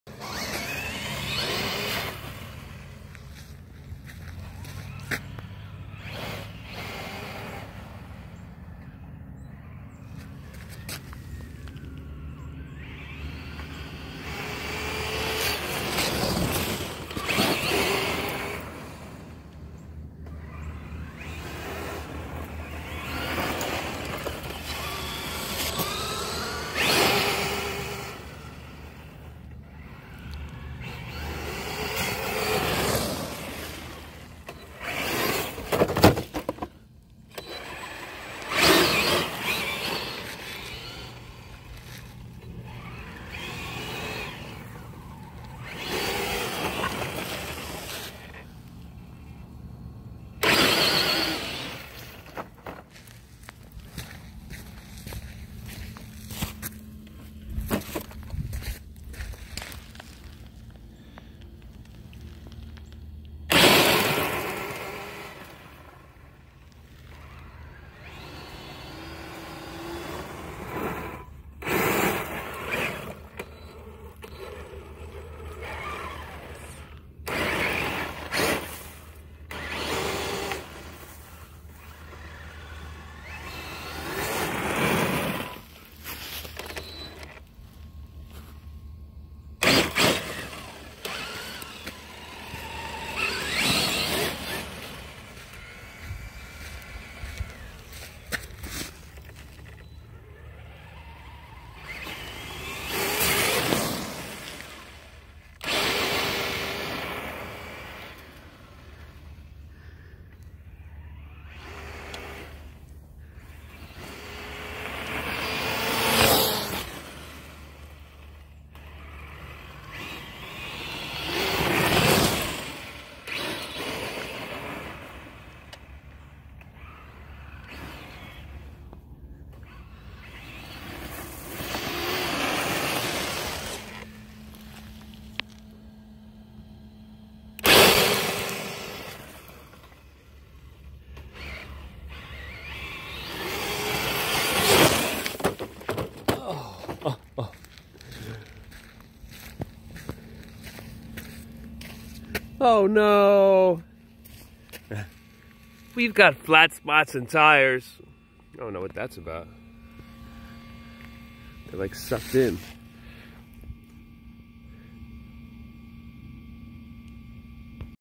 full throttle through puddles "arrma" brushless Trx4!!!!
guys, if you have a Trx4 or any crawler for that matter. you have to try throwing a brushless (non crawler) system in it. Even if it's for one pack. It's just so hilarious and fun because crawlers aren't supposed to go fast and it looks really funny! 🤣🤣😆 I don't know why but it's so enjoyable!!! love you guys🤙